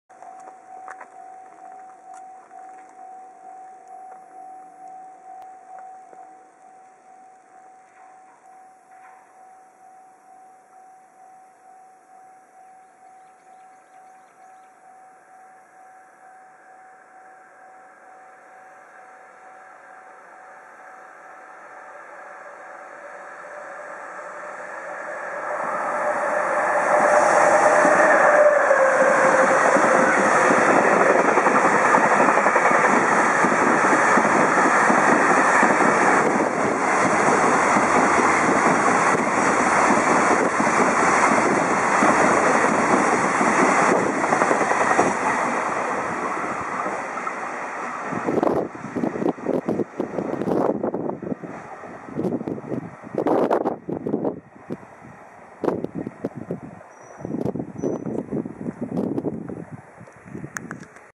58レ 全国通運 スーパーグリーンシャトルライナー JR貨物 EF66 107号機 吹田機関区所属 代走牽引高速貨物列車 東海道本線 追分踏切 清水駅 草薙駅間通過
2014年5月24日 午前AM4時03分頃通過。安治川口駅発東京貨物ターミナル駅行き